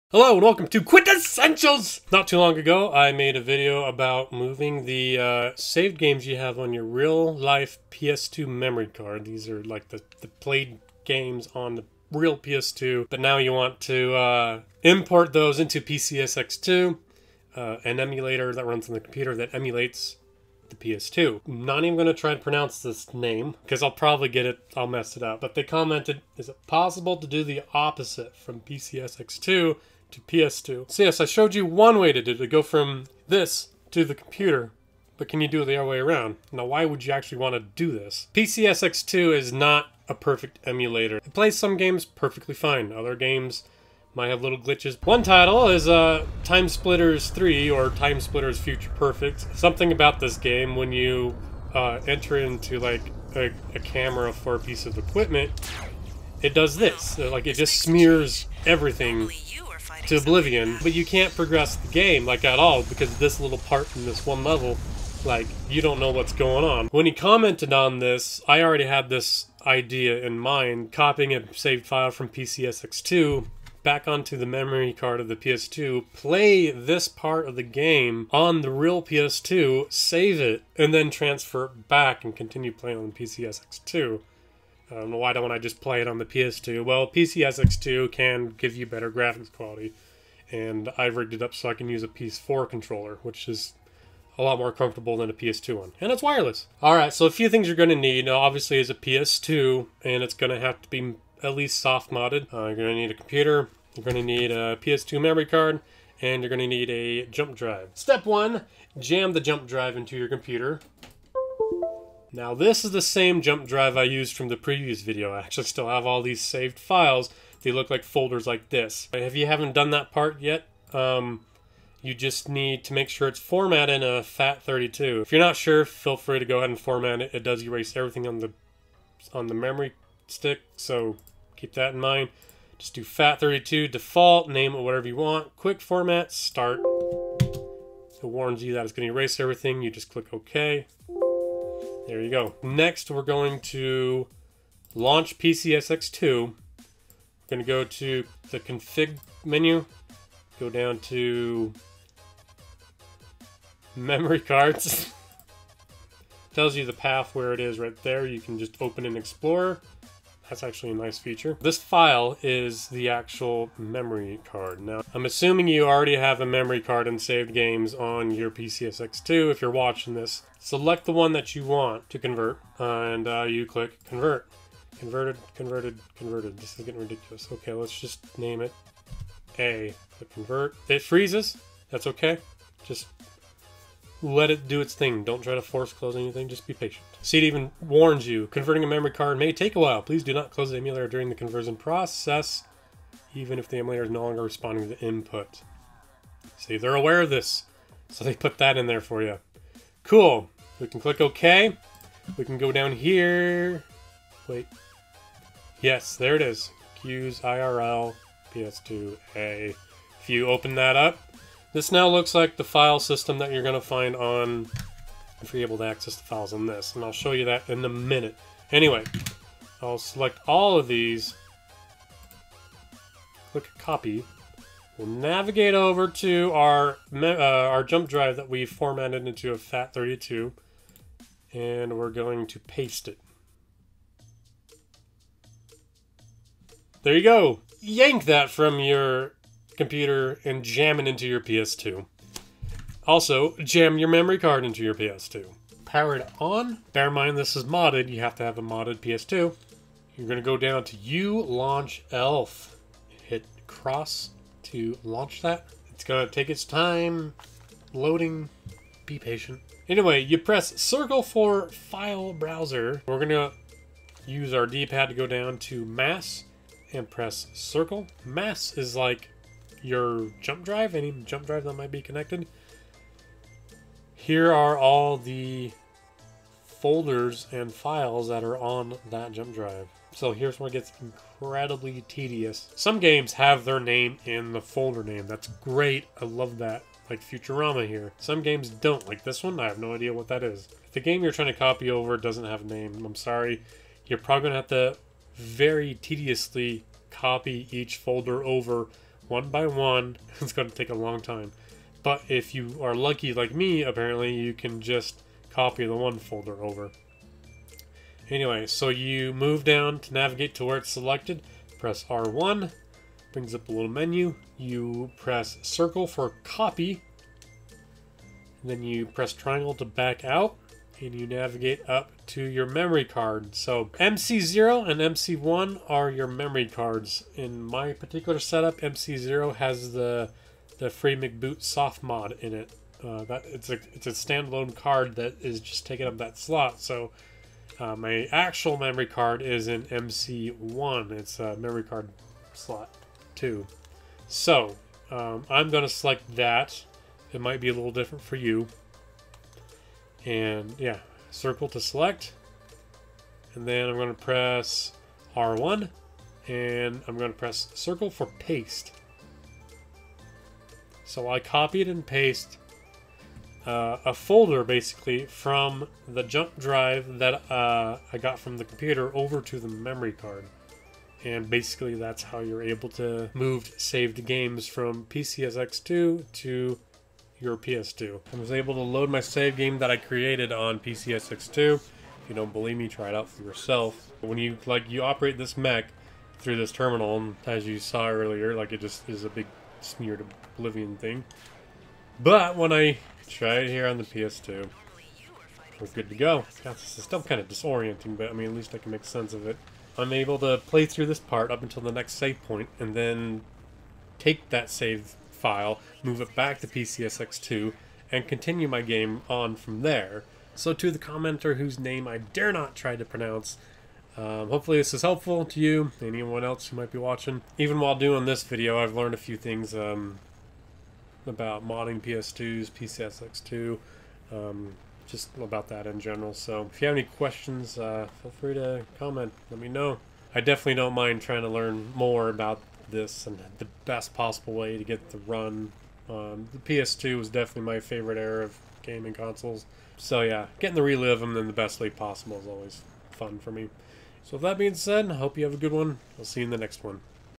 Moving PS2 Save Files From PS2 Emulator to Real PS2
I show how to move a game save file from PCSX2 to an actual PS2 so that you can start a game on PCSX2 and then continue it on a PS2 without having to redo all your progress.